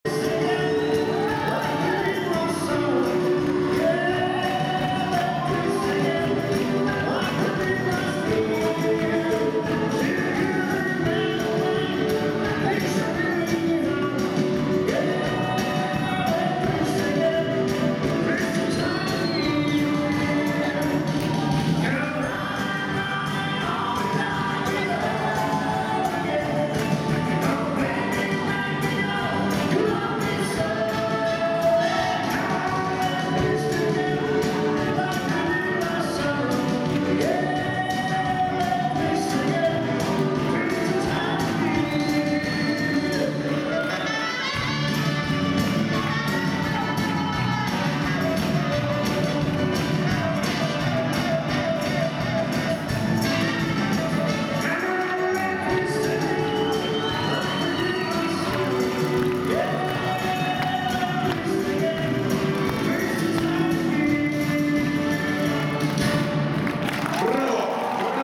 Gusev Andrey - Cherevichnaya Elizaveta, jive
Гусев Андрей - Черевичная Елизавета, jive
Слава России - 2011 WDSF International - La
Russian Open 2011 Moscow
video by Victoria Gorbacheva